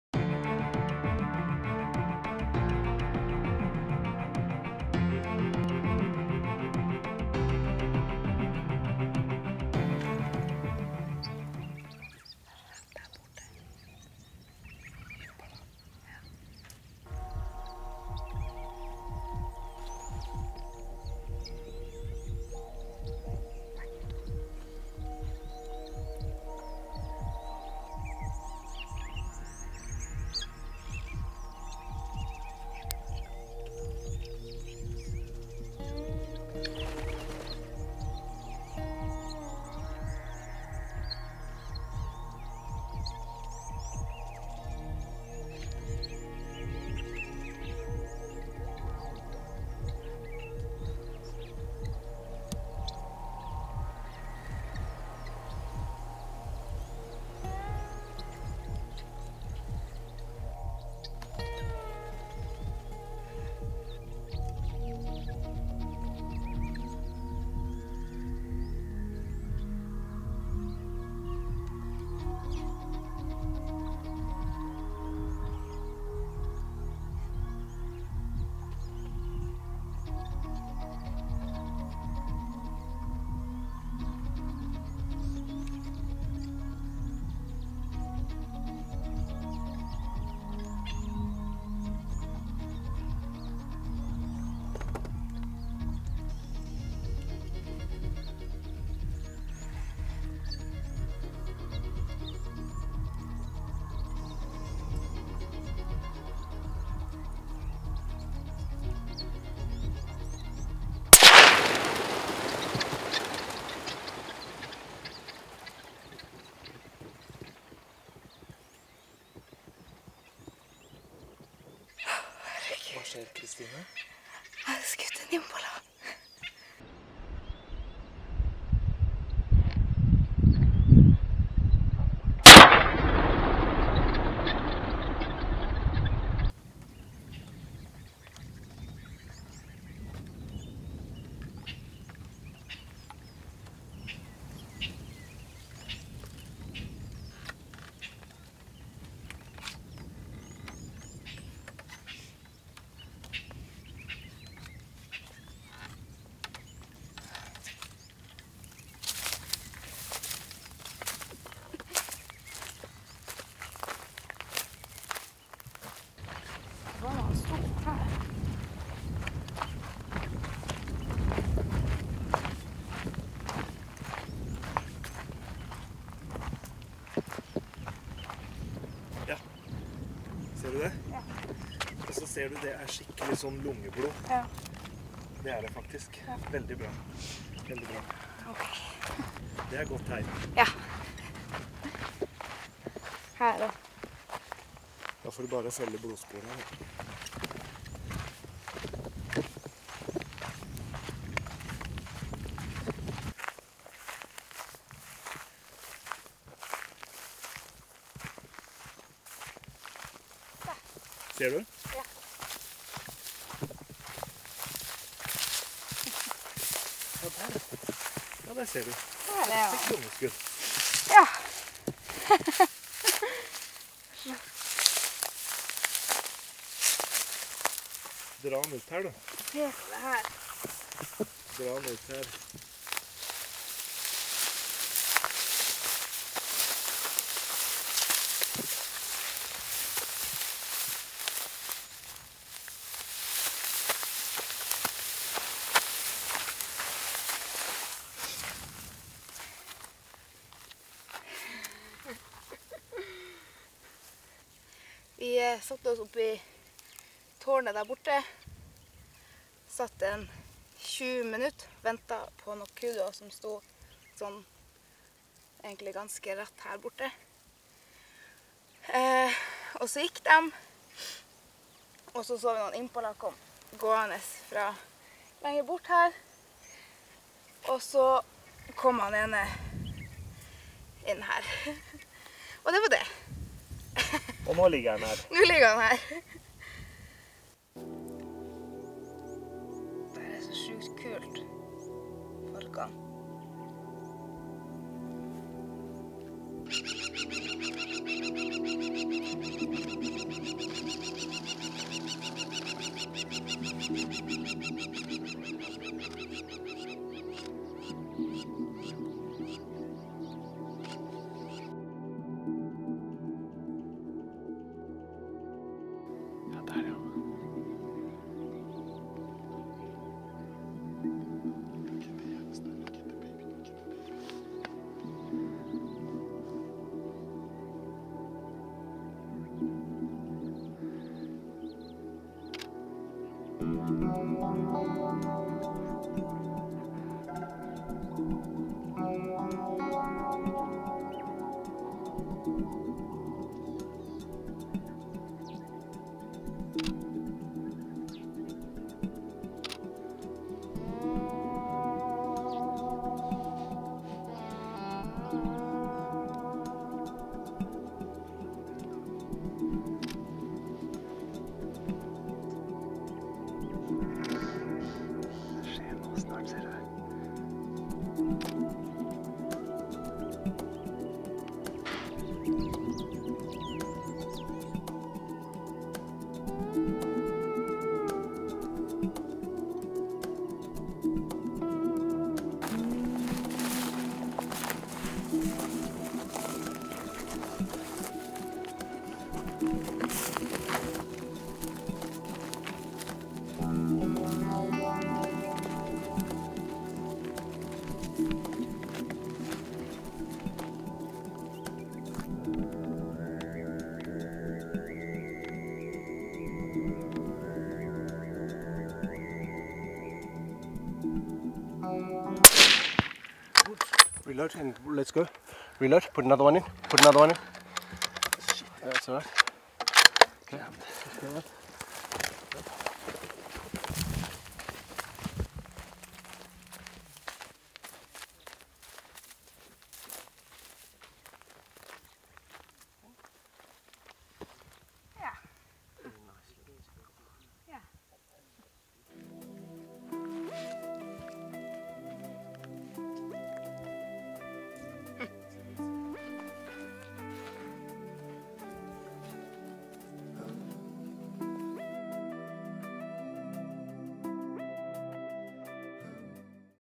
Hunting in Africa with Kristine and Kristoffer
In this video we follow Kristine Strøm and Kristoffer Clausen hunting in Botswana on Kgwebe hills.